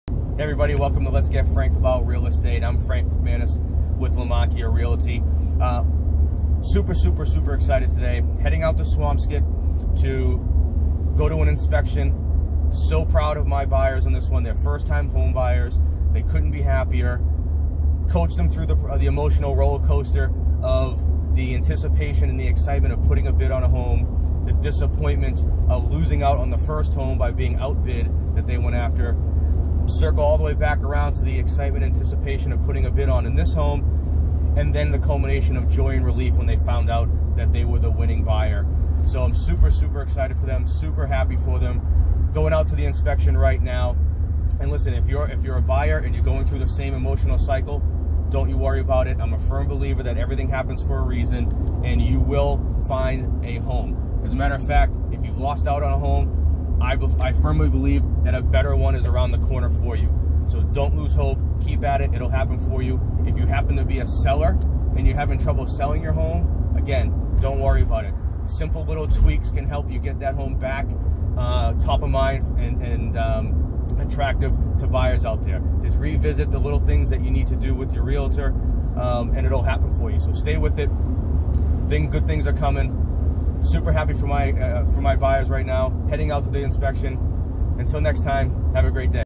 inspection day
Getting through the emotional roller coaster and becoming a home owner